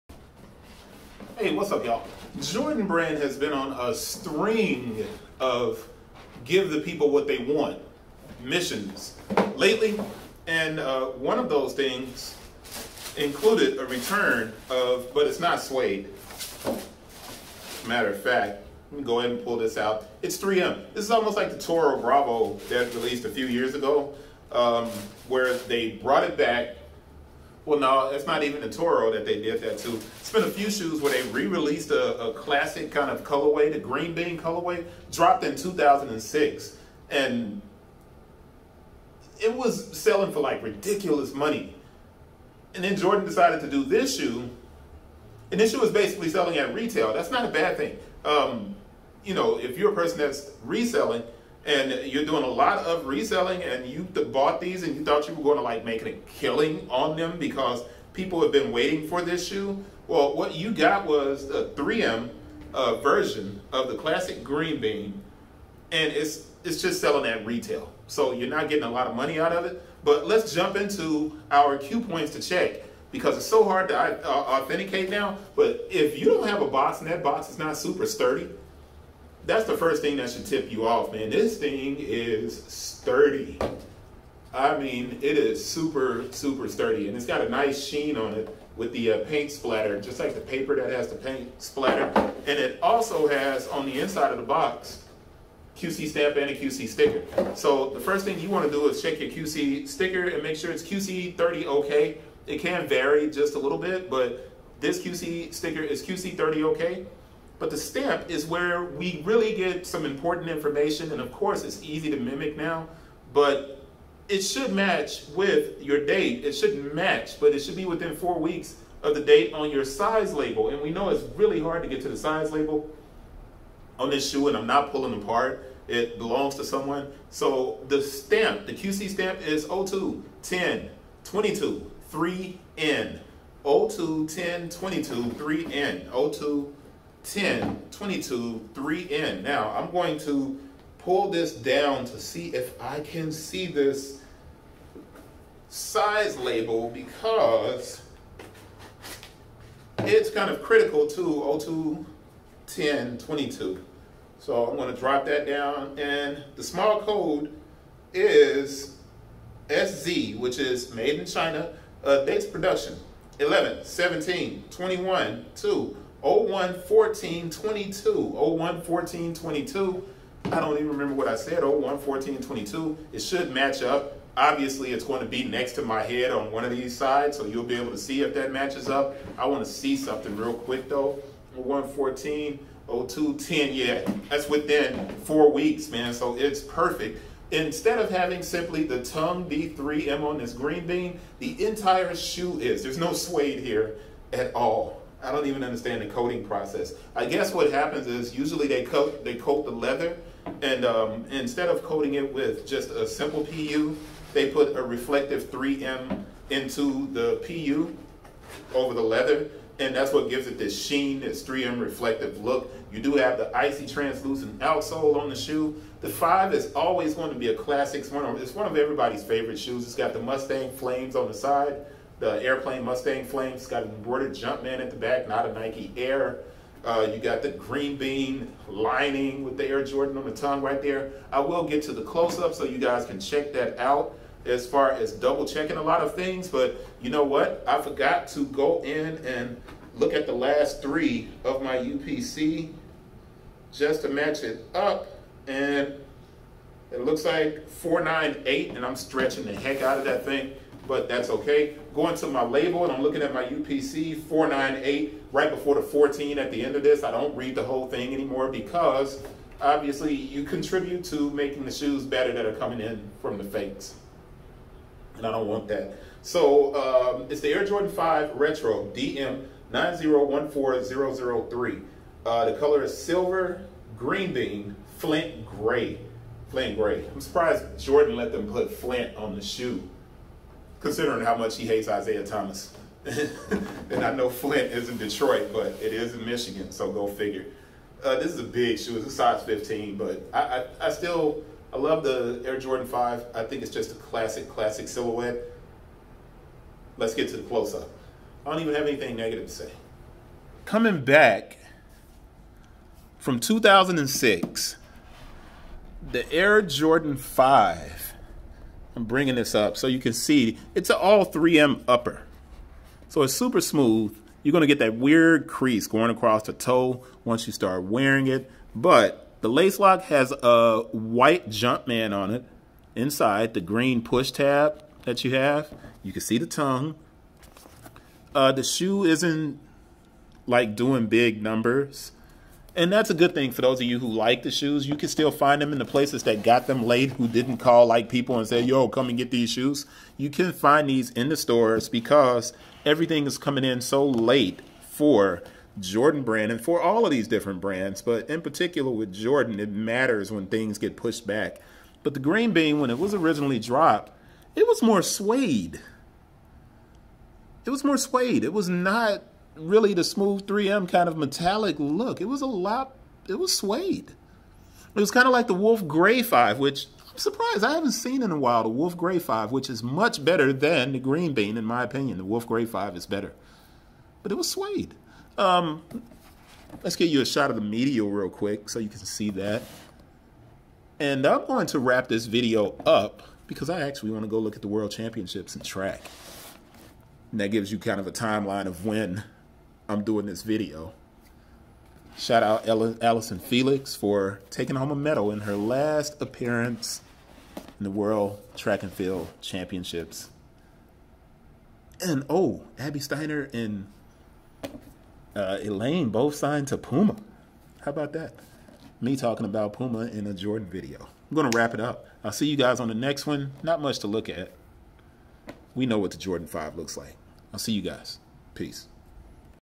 Air Jordan 5 Retro DM9014-003 Silver/Green Bean-Flint Grey
The Green Bean dropped in 2006 and returns in 2022 featuring the same 3M reflective upper and green accents. The shoe has a translucent outsole with a clear lacelock featuring a green with white Jumpman logo on the springloaded lacelock.